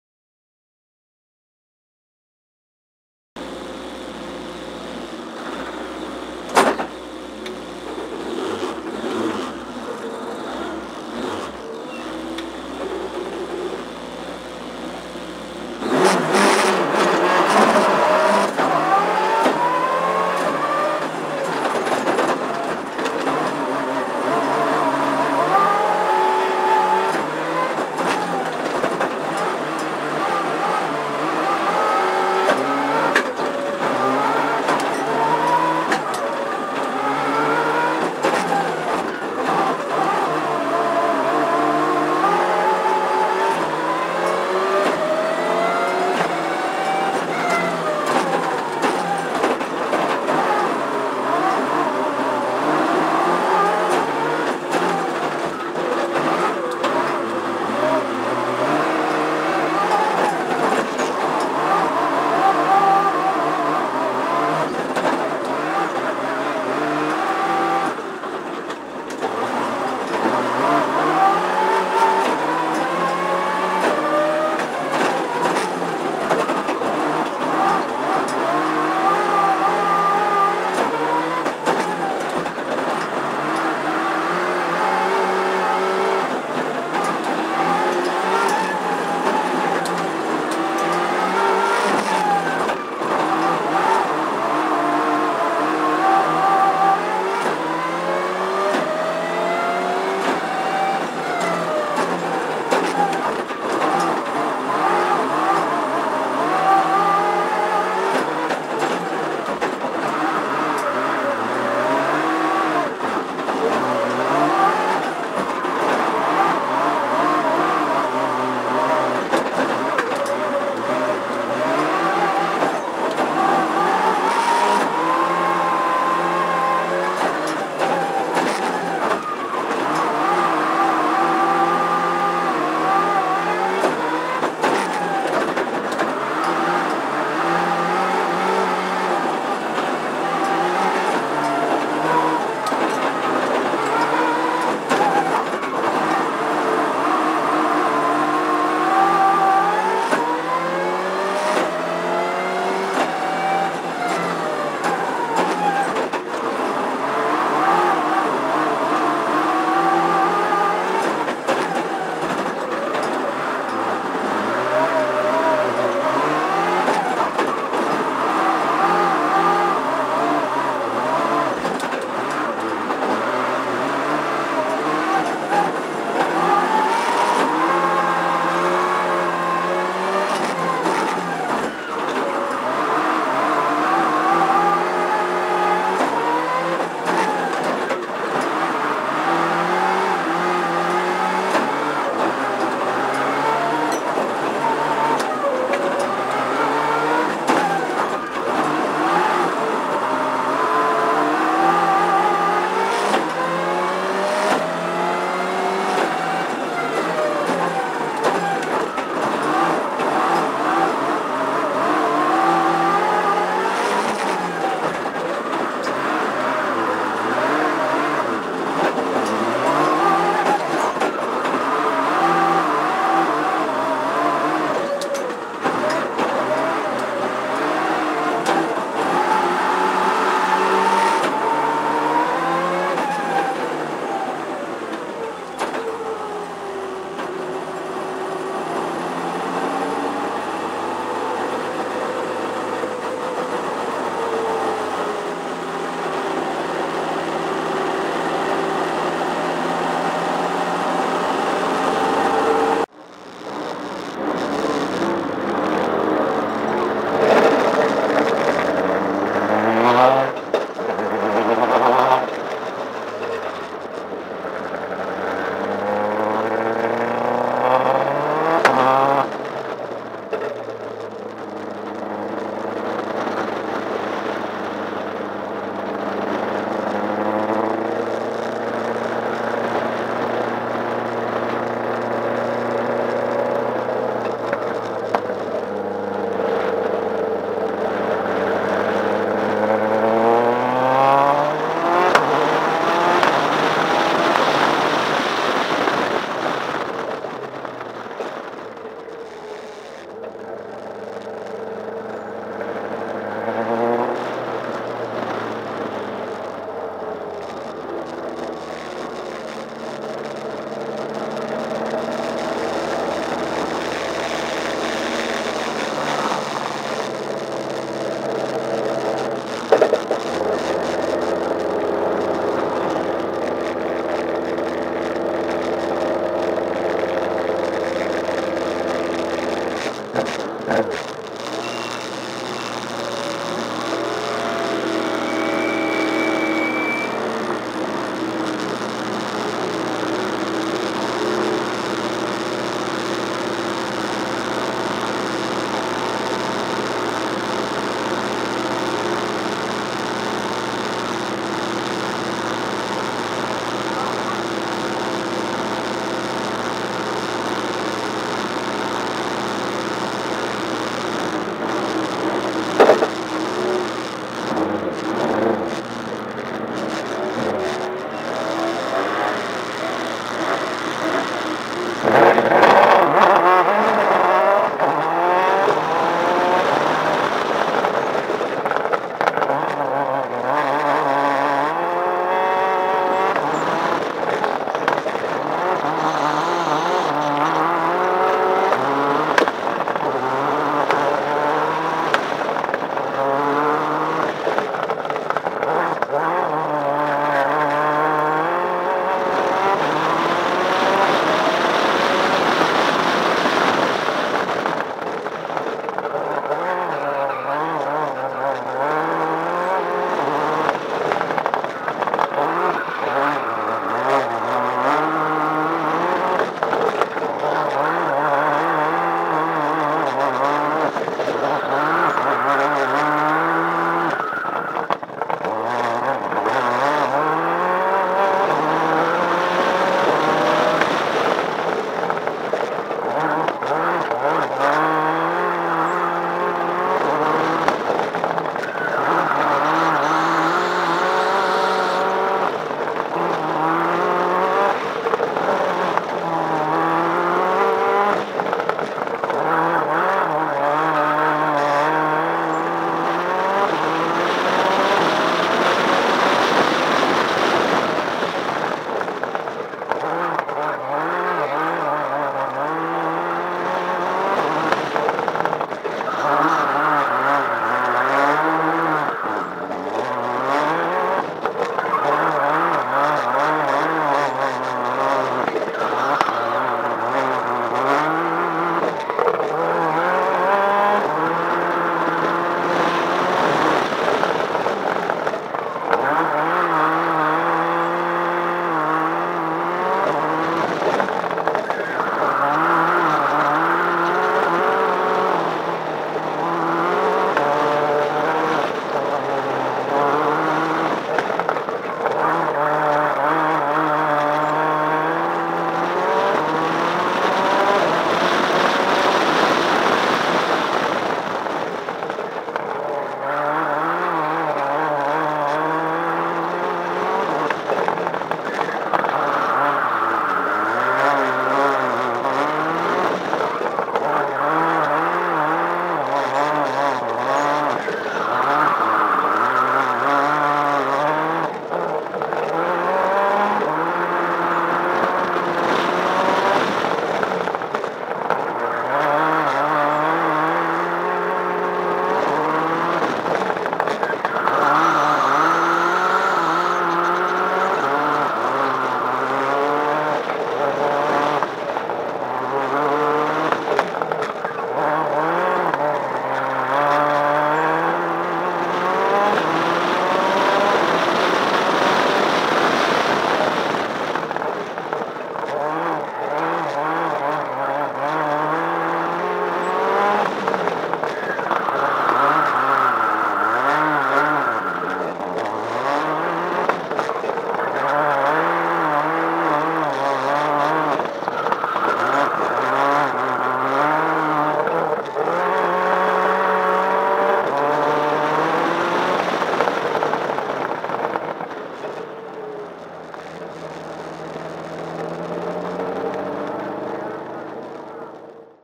Tonino "Tony" Marchetti su Ford Focus WRC
Formula Challenge "Happy Valley", Pinarella di Cervia